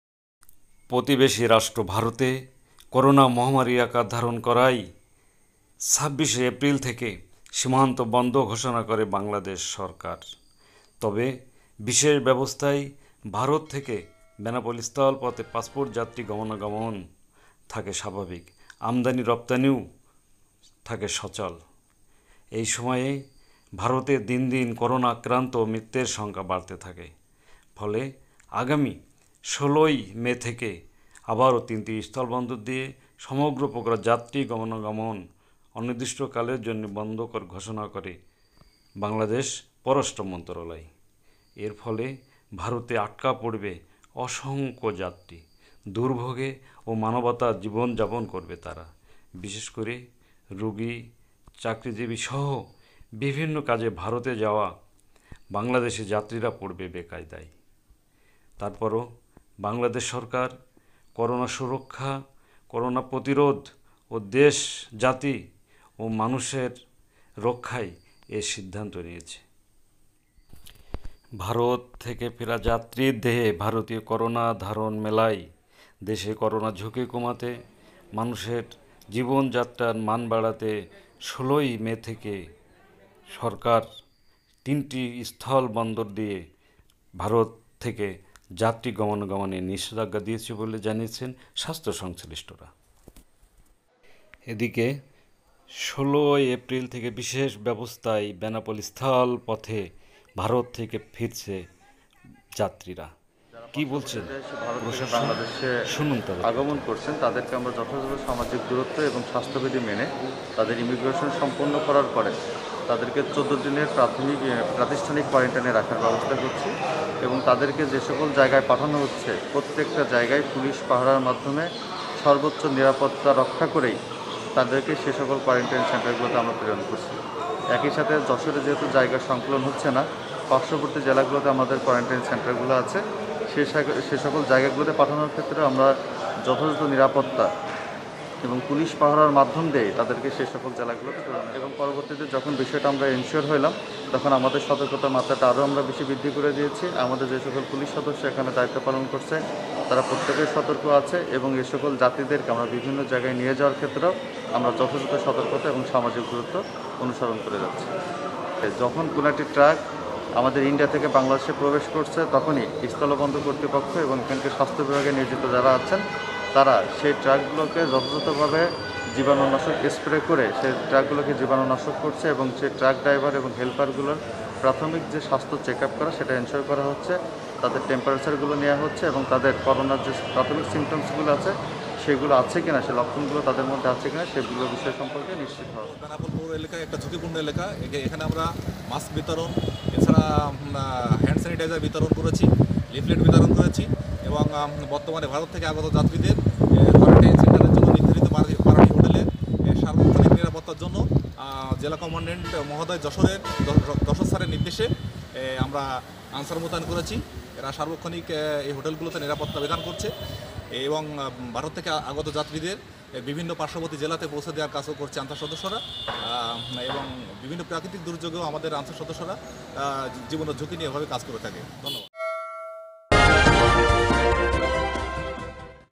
ভারত থেকে যাত্রী প্রবেশে আবারও নিষেধাজ্ঞা।৩টি সীমান্ত বন্ধ।
১৬মে থেকে ৩টি সীমান্ত বন্ধের নির্দেশনা দিয়েছে পররাষ্ট্র মন্ত্রনালয়। করোনা ঝুকি কমাতে ২৬এপ্রিল থেকে সীমান্ত বন্ধ ষোষনা করা হলেও বেনাপোল বন্দর দিয়ে বিশেষ ব্যাবস্থায়  দেশে ফেরে যাত্রীরা।বাড়তে থাকে করোনা ঝুকি।।দেখুন ভিডিওতে লাইক শেয়ার ও কমেন্ট করে জানান আপনার মতামত